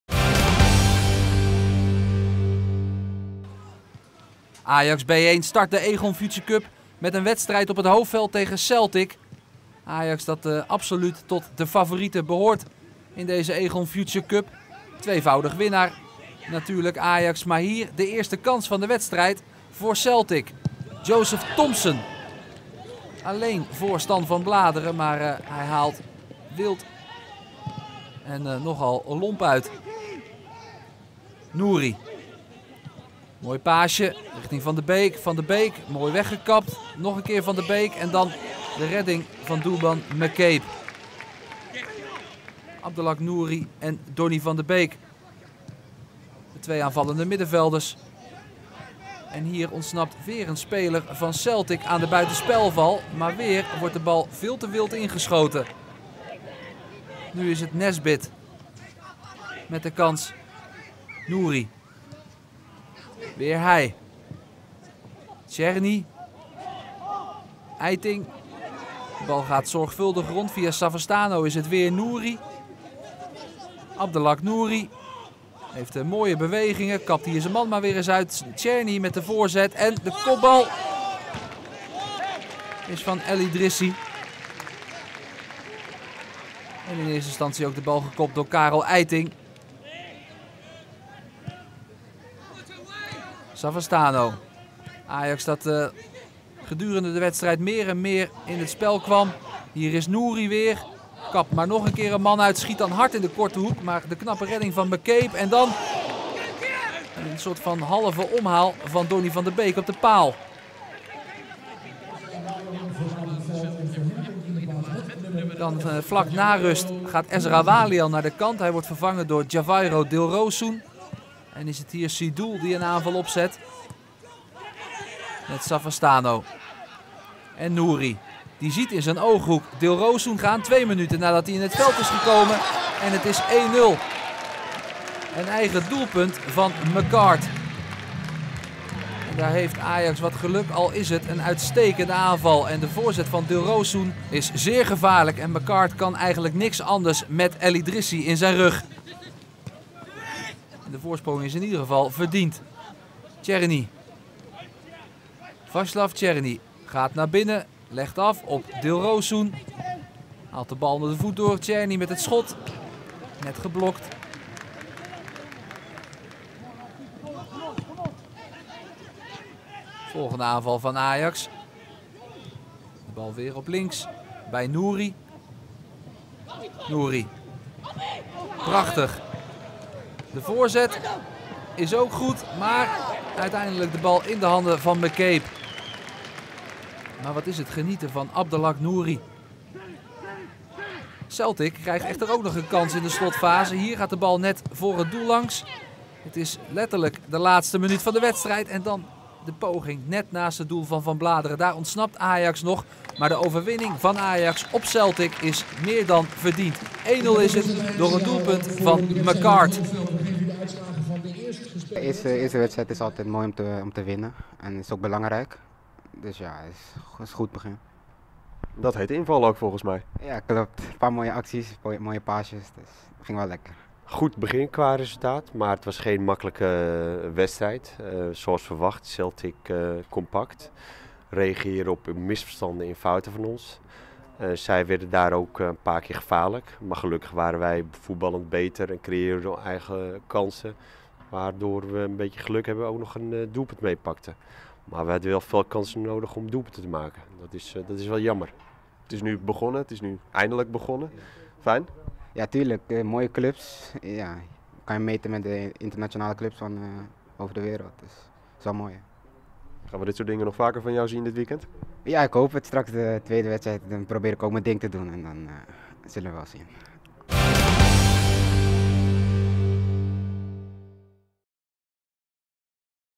Ajax start met overwinning op Celtic
Ajax B1 is de Aegon Future Cup uitstekend begonnen. De ploeg van Saïd Ouaali won door een eigen doelpunt met 1-0 van Celtic.